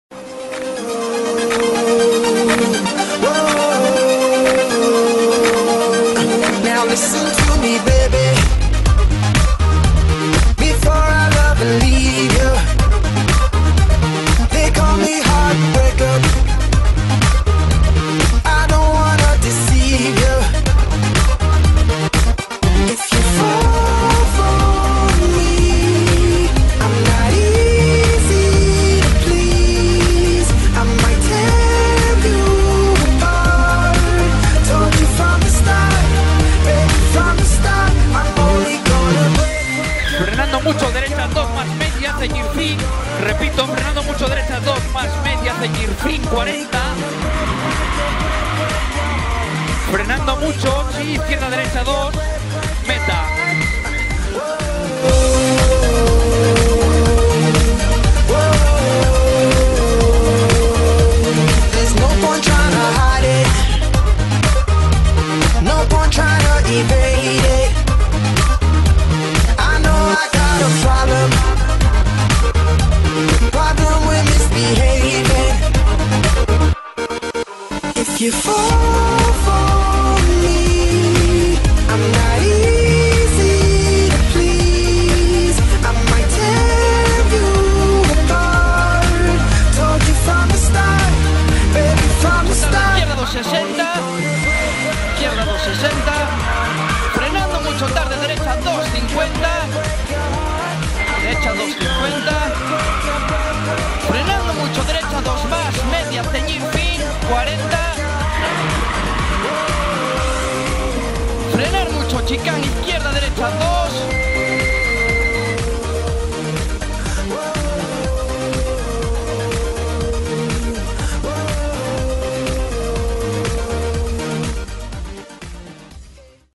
Cursillo de Copilotos- Pravia 2010
Resumen musical del Cursillo de Copilotos realizado en Pravia e impartido por Daniel Cue, Gabri Suarez y Jose Antonio Suarez "Cohete"